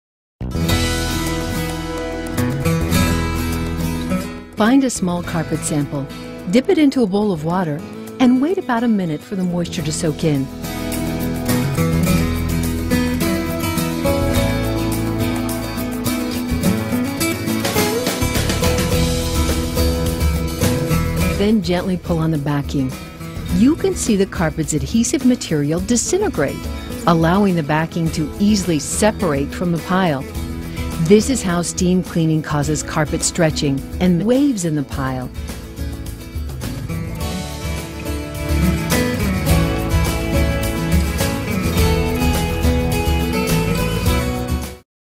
SEBO duo P Wet Carpet Demo
Made in Germany, SEBO's duo Cleaning System brilliantly cleans carpets in large or small areas, and it effectively removes all types of spots and stubborn stains from upholstery and carpets.  The duo Brush Machine and cleaning powder are designed for large carpeted areas, while the duo Daisy, the Hand Brush and powder are designed to clean smaller areas, such as stairs, entryways, hallways, spot stains, automobiles, RVs and boats.  This do-it-yourself system is highly effective, easy to use, saves time and money, and avoids the potential carpet damage caused by some wet cleaning methods.

The advantages of using the duo dry-cleaning system are the elimination of dust mite allergens, no need to move furniture, no detergent residue, a built-in fiber
protector that helps to prevent future stains and soiling, and the ability to use the carpet or upholstery within 30 to 60 minutes after cleaning is completed.

The duo Cleaning System consists of the following four components: 1)  duo-P Carpet and Upholstery Cleaning Powder, 2)  duo Brush Machine, 3)  duo Daisy Brush and 4) duo Hand Brush.